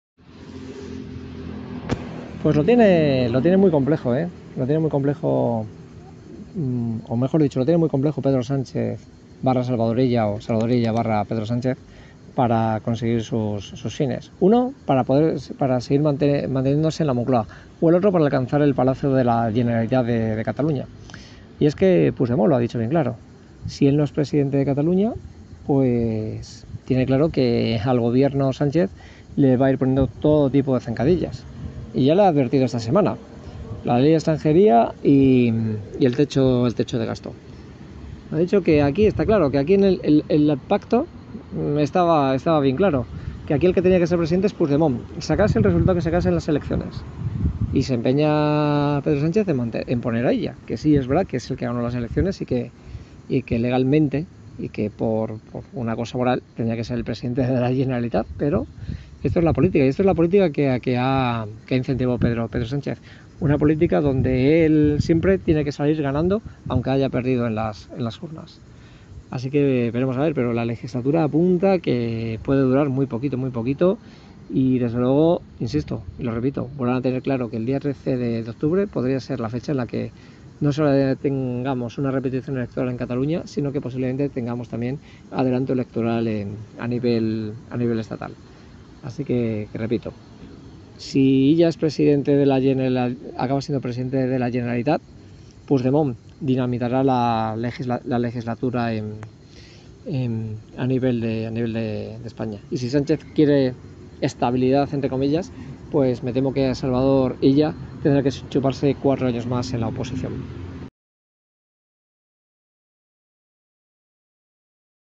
Illa y Puigdemont tienen cogido por las criadillas a Sánchez
El socialista y el independentista están metiendo en un serio brete al presidente socialista a la hora de saber quien gobernará en Cataluña y en La Moncloa.